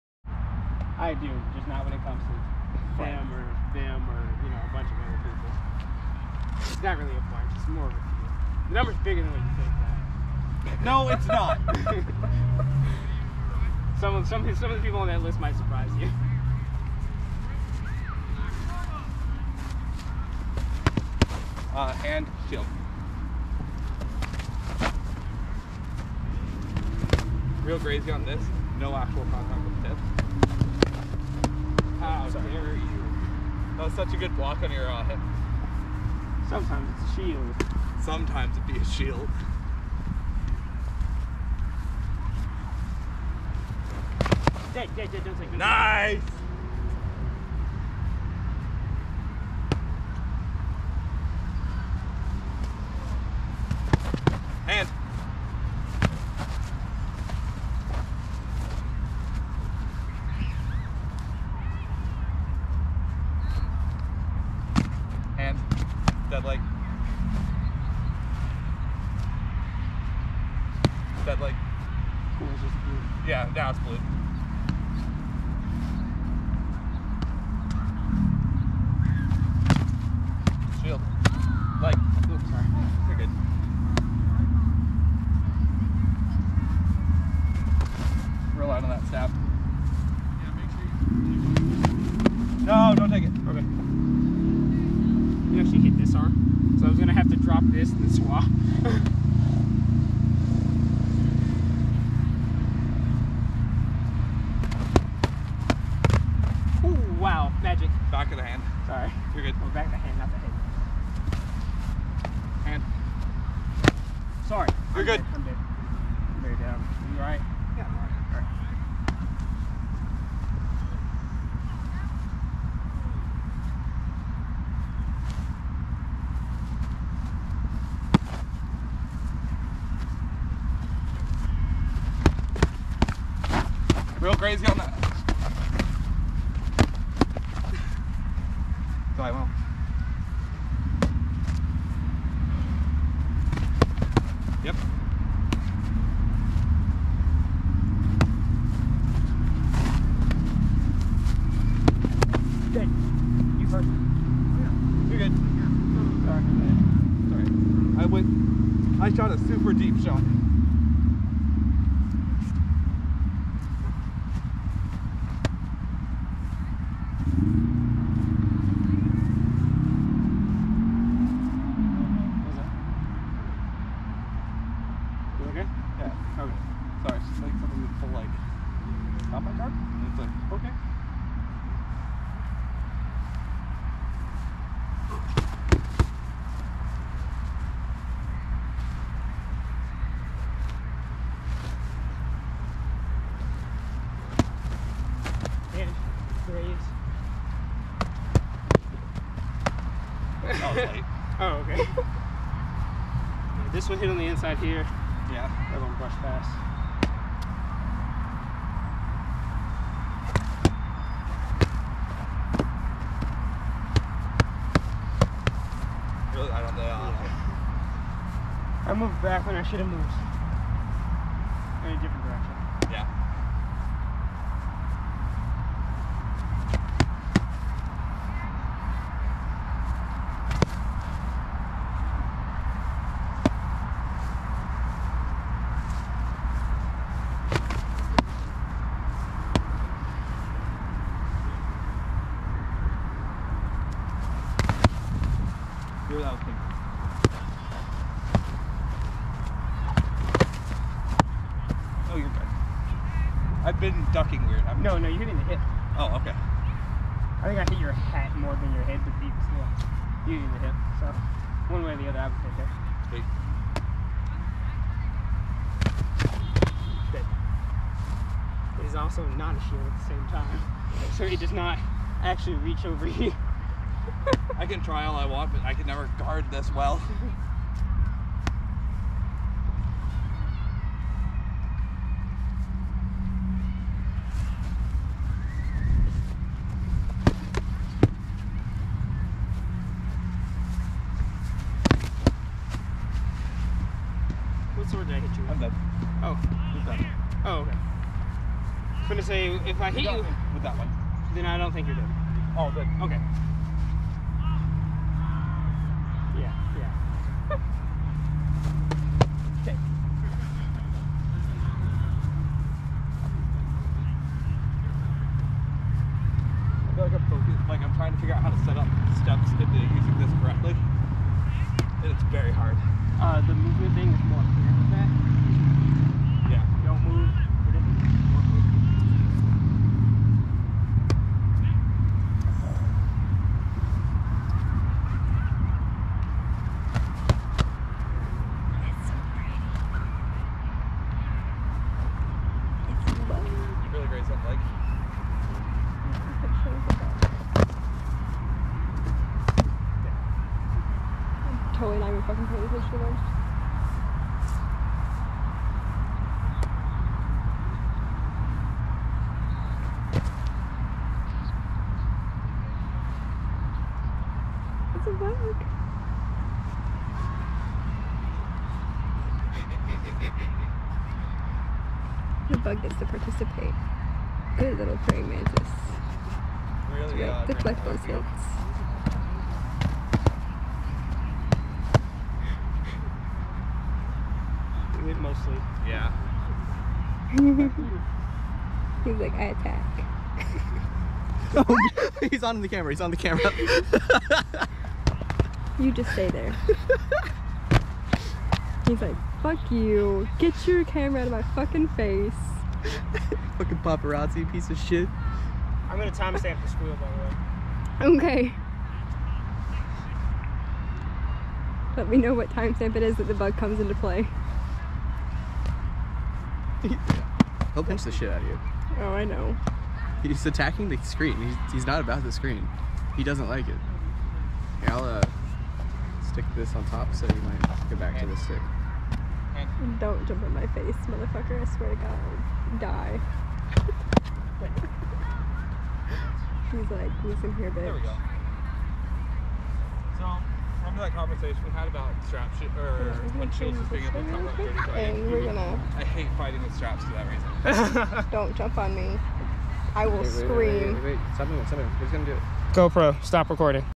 Talon vs. Ink Spars 11/7/21
We're using the Belegarth rule set.

6:28 - Praying Mantis Style